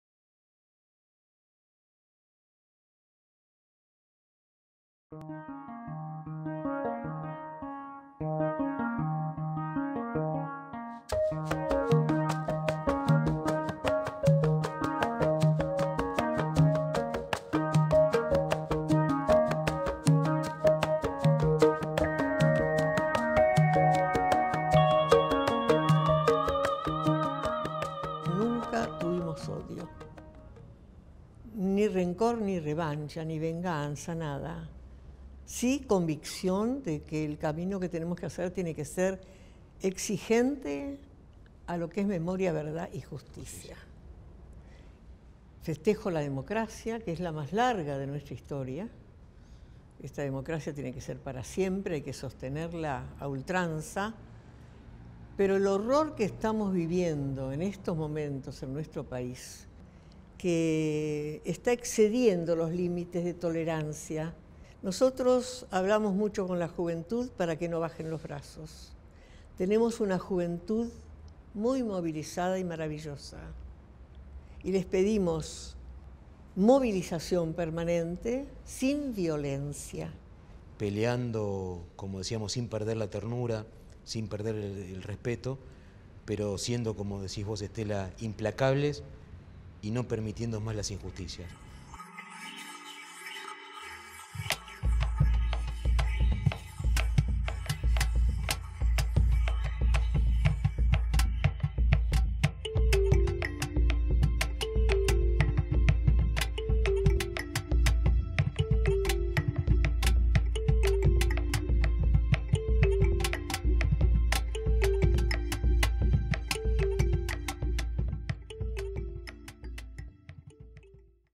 MEMORIA VERDAD Y JUSTICIA - Conversación con Estela de Carlotto
MEMORIA VERDAD Y JUSTICIA🎞📽🙌🏻

Compartimos un fragmento de la charla entre Estela de Carlotto, titular de Abuelas de Plaza de Mayo, y el secretario general de la FEDUN, Daniel Ricci.

Un repaso por la lucha de las Abuelas, los derechos humanos, la educación, la actualidad política y muchos temas más que pueden ver en nuestro canal.